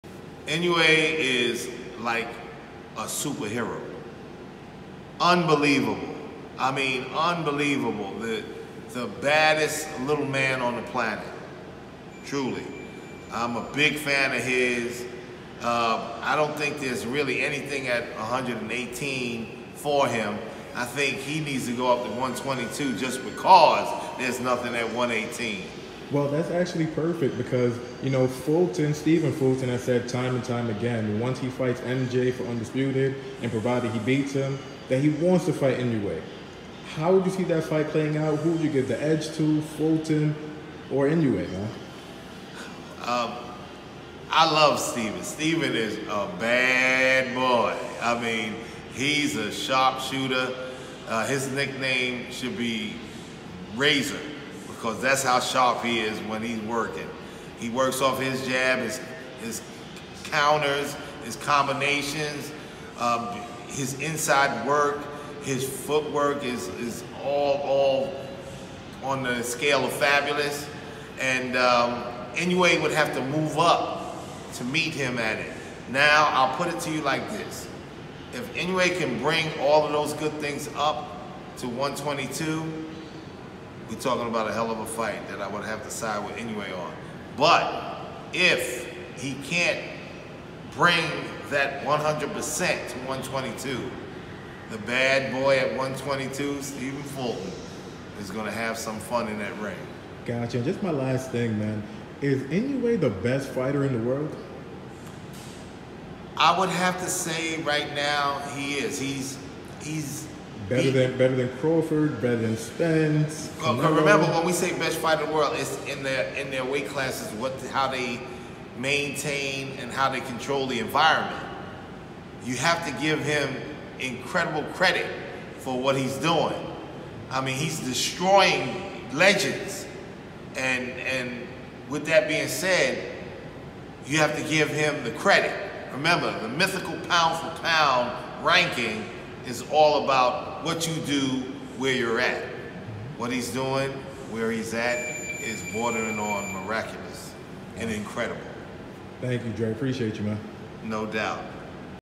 Andre Rozier Praises Naoya Inoue, Calls Him The World’s Best Fighter
Naoya Inoue, in Andre Rozier’s opinion, has officially stamped his place as the best fighter in the world